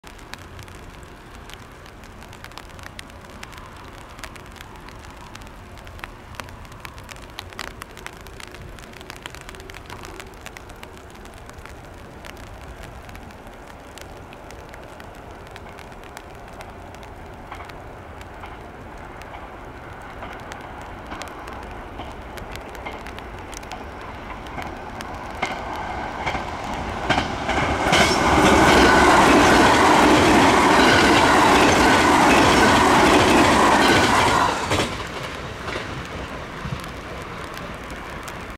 2021/02/08 5084M 特急 まいづる10号+はしだて4号 287系(FC04編成+FA06編成)
JR山陰本線 千妻踏切